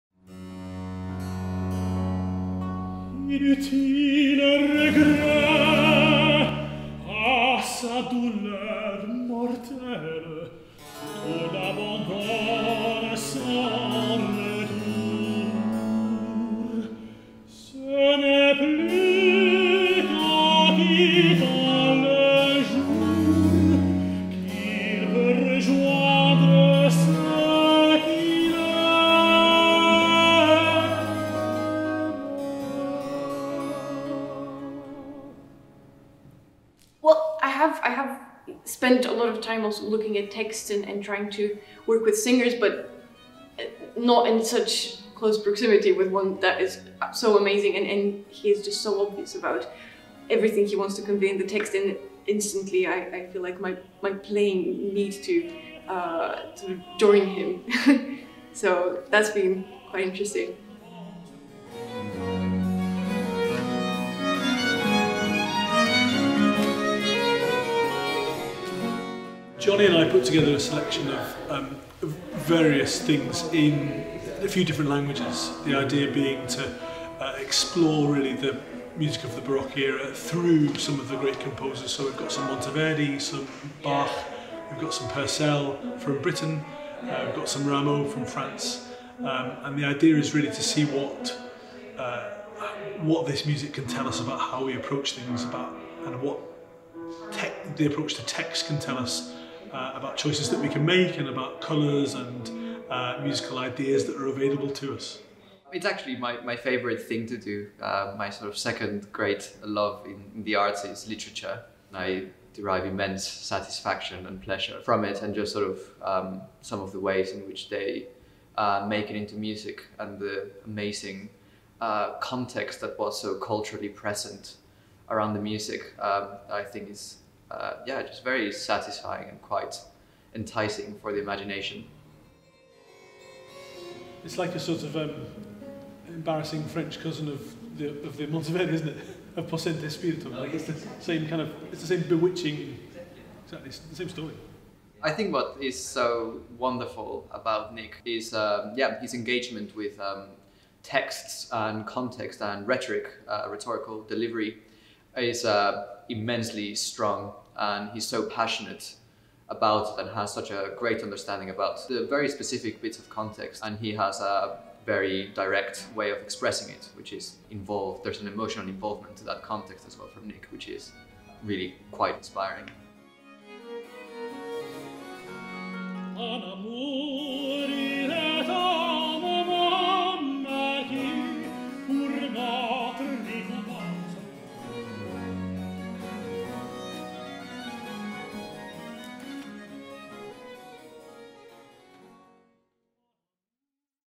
Arcangelo New Ensemblists | Video Diary #3
Launched in Arcangelo's 10th Anniversary Season, the Arcangelo New Ensemblists scheme provides an innovative and wide-ranging programme of experience, training and development to outstanding young instrumentalists specialised in ensemble period performance.

Sophia Prodanova (Violin), Sergio Bucheli (Lute) and Ismael Campanero Nieto (Bass) are the first three Arcangelo New Ensemblists, for the period 2020-22.

In this third video diary, Sophia and Sergio work on accompaniment of voice and text with Arcangelo's Artistic Director Jonathan Cohen and guest mentor Nicholas Mulroy. The session forms part of a sequence of academy days scheduled throughout the programme.